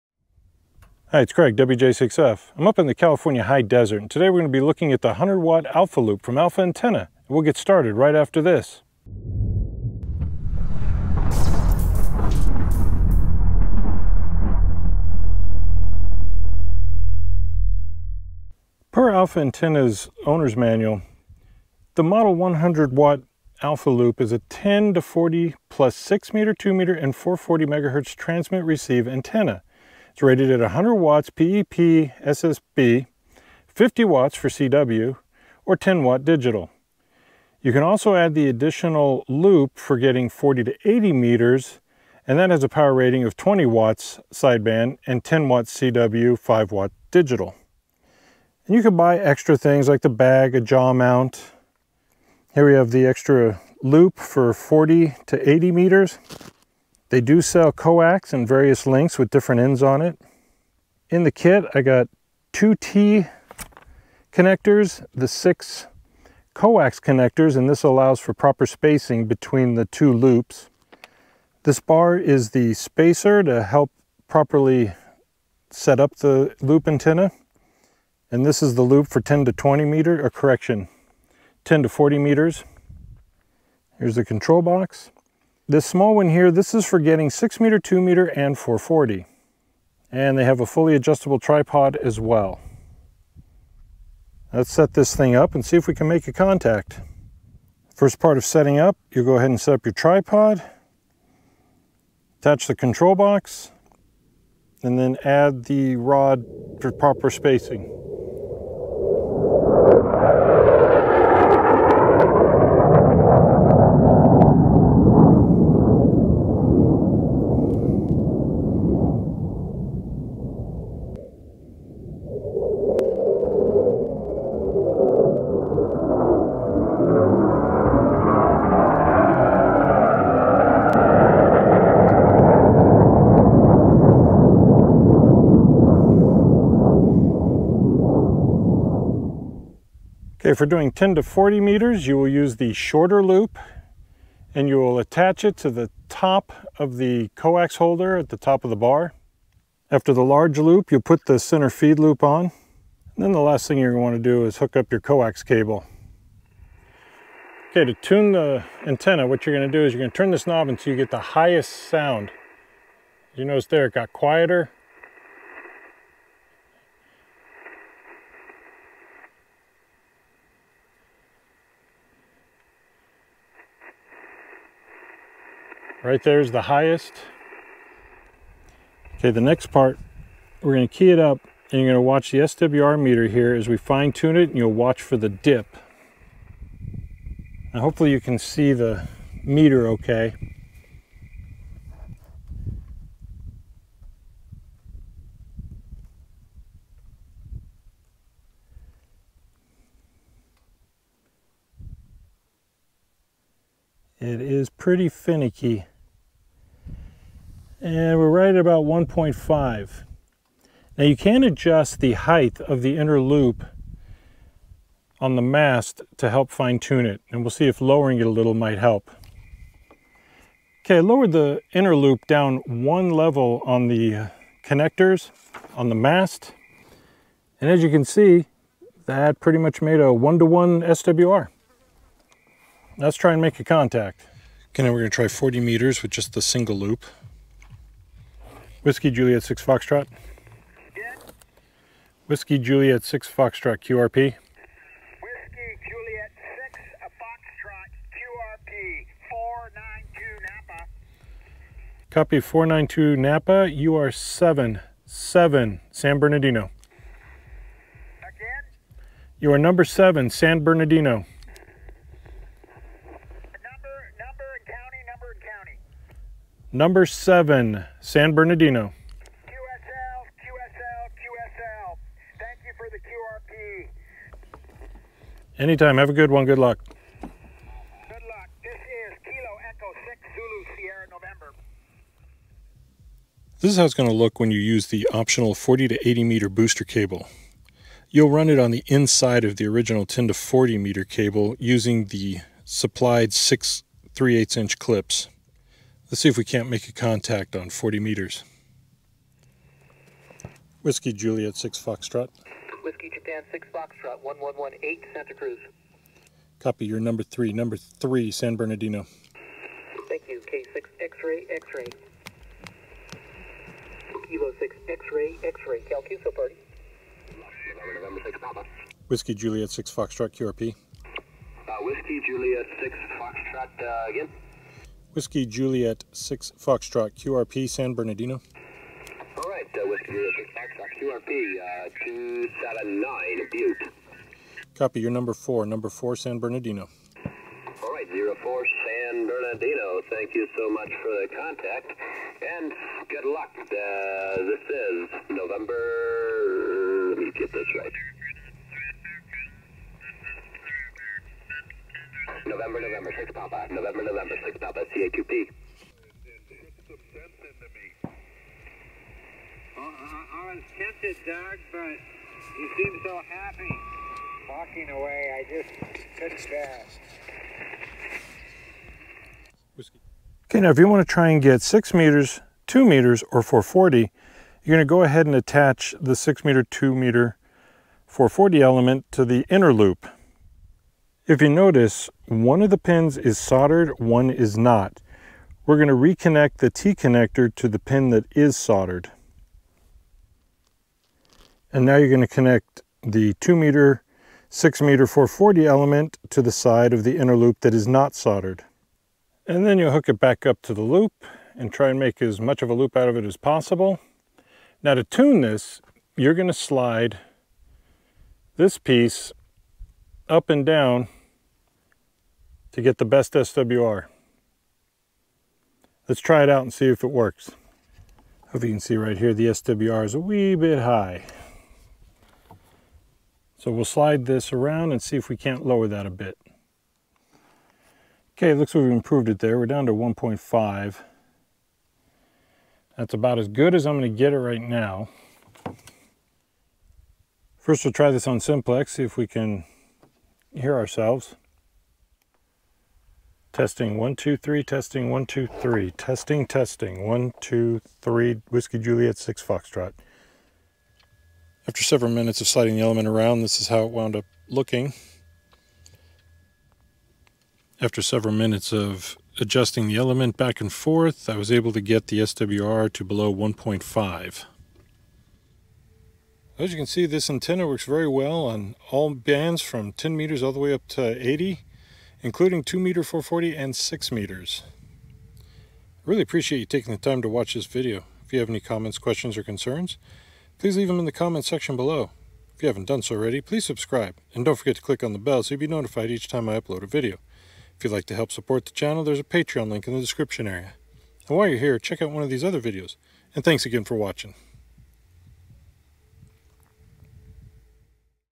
100 Watt Alpha Loop From Alpha Antenna 10-80M HF MagLoop
In this video I will show you the 100 Watt Alpha Loop from Alpha Antenna. Per the Alpha Antenna website; "Portable HF Magnetic Loop MagLoop antenna with coverage of 10-80 Meters plus an option for a 6 Meter, 2 Meter & 440 MHz loop handles 100 Watts PEP SSB on 10-40 Meters and VHF UHF. Install the included Booster Cable and enable 20W PEP SSB on 40/60/80 Meters for this MagLoop."

~~~CHAPTERS~~~
0:00 Intro
0:18 Parts Are Parts & Info
1:37 Setup
2:37 HF Tuning
3:55 Making Contacts
6:09 6m, 2m, 440 Setup & Tuning
7:45 Conclusion